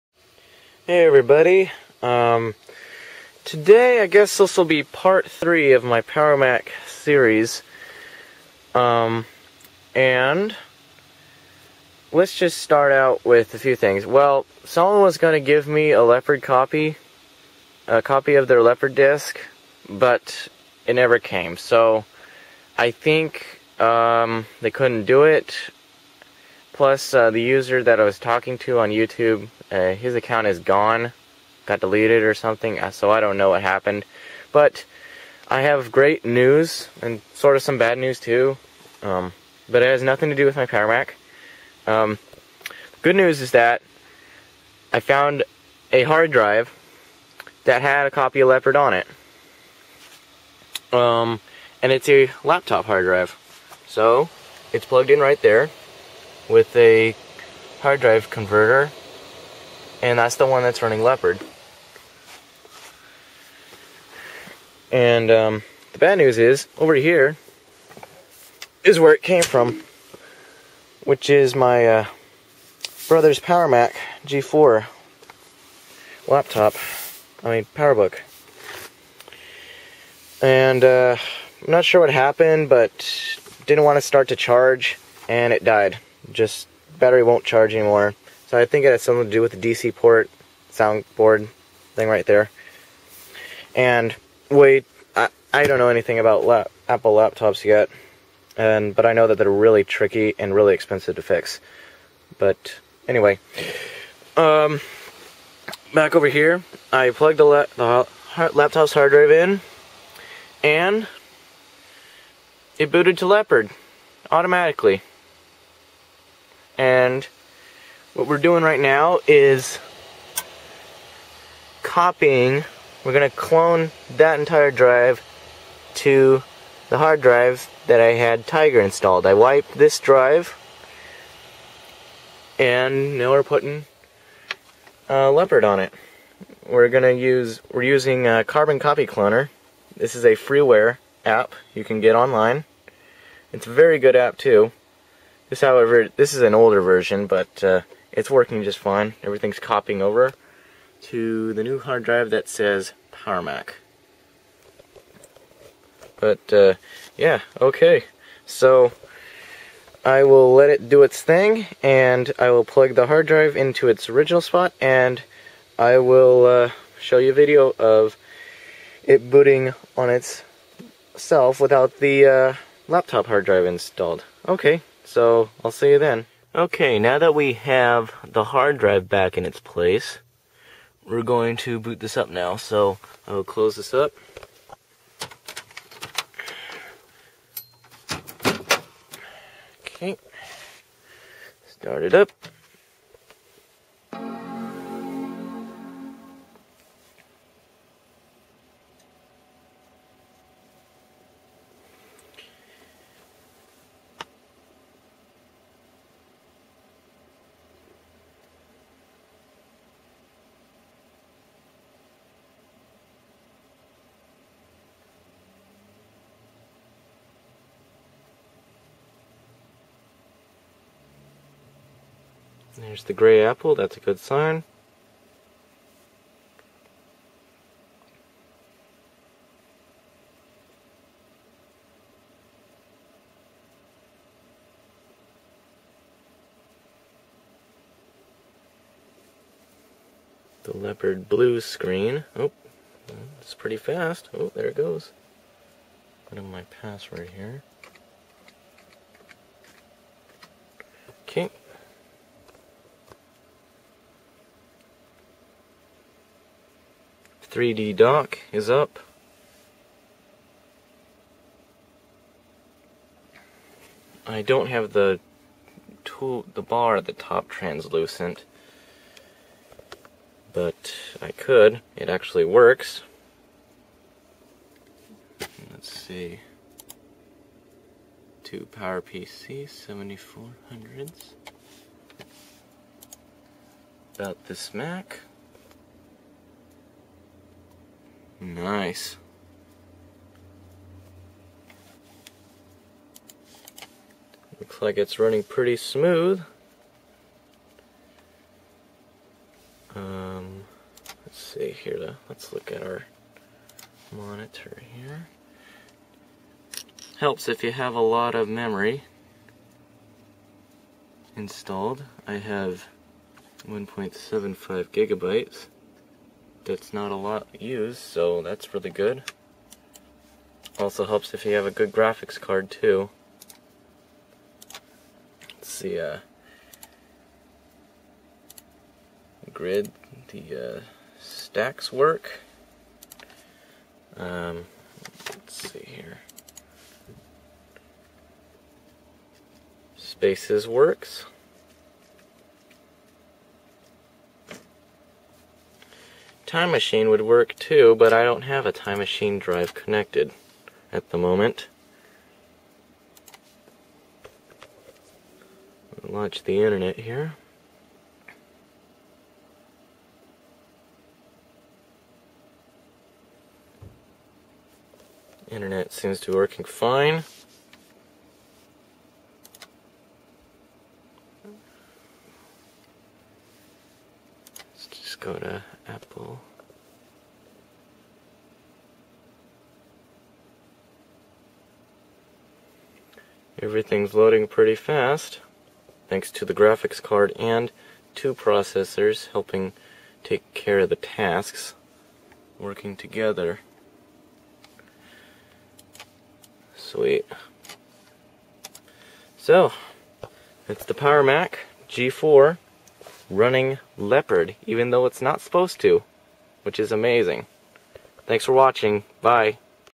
Power Mac G4 Leopard Install (Part 3)
I got my PowerMac G4 Sawtooth to run Mac OS X Leopard!
I didn't receive a disk someone was going to send me so instead I used my brother's PowerBook G4 HD that had Leopard on it already and I cloned it with Carbon Copy Cloner. Everything works great and flawlessly. It feels a bit snappier than Tiger but might not do well with flash content on the web. It probably could use a Dual 500Mhz G4 to make it feel faster. Overall, best upgrade I've ever done to it.

(Technically, this isn't a "true" install, but this method is common and works when you don't have a disk.)

Specs: Dual 450Mhz G4,1.75 GB Ram, ATi Radeon 9800 Pro Mac Edition, 120 GB HD, SuperDrive, USB 2.0 5-port Card, Mac OS X Leopard Installed.